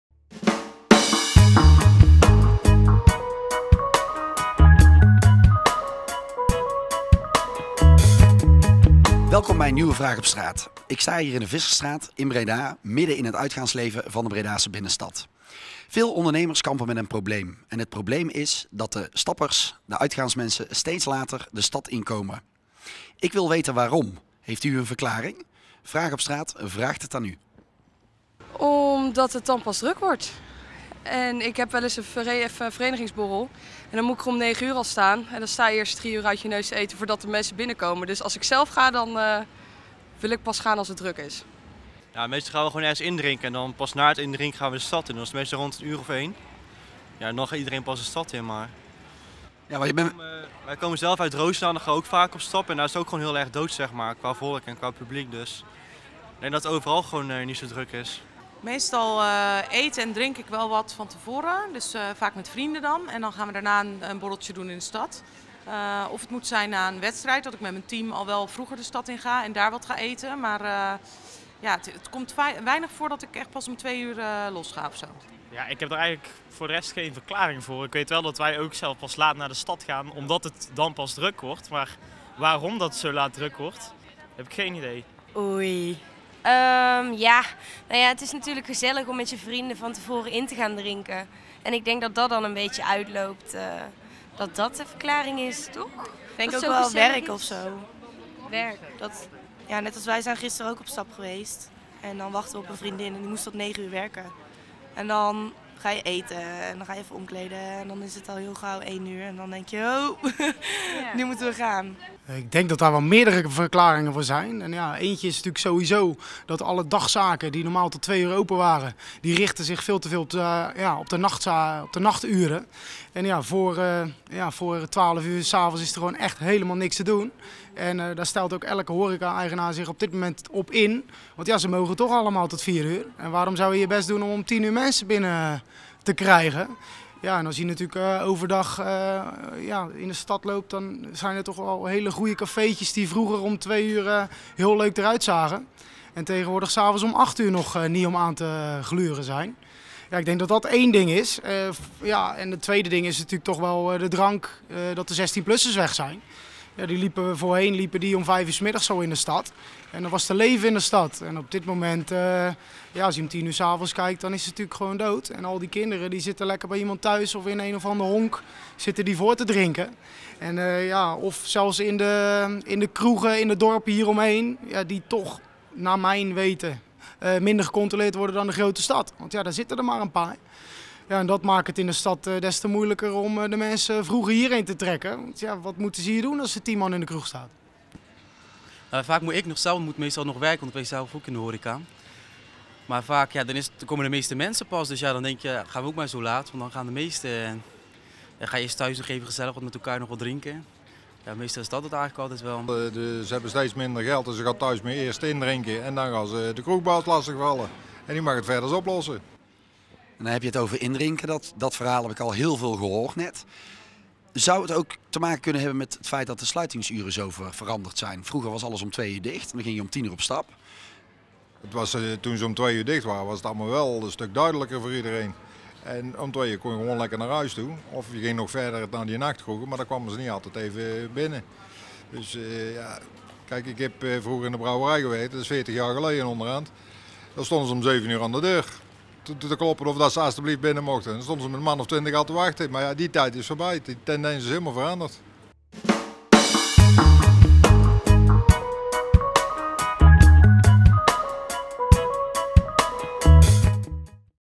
Vraag op Straat: Steeds later op stap
We vragen waarom de mensen steeds later op stap gaat.